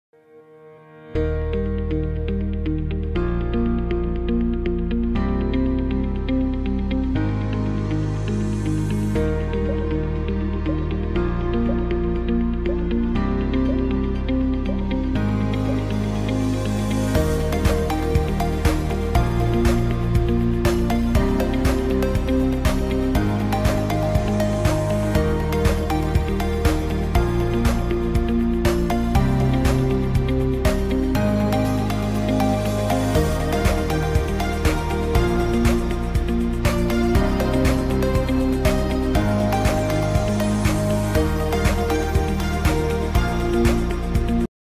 Torres @ Hong Kong CNY Cup (Day 2) [Highlights]
Torres @ Hong Kong CNY Cup (Day 2) [Highlights] 7/2/2019
Sagan Tosu vs Shandong Luneng
Torres: 1 Goal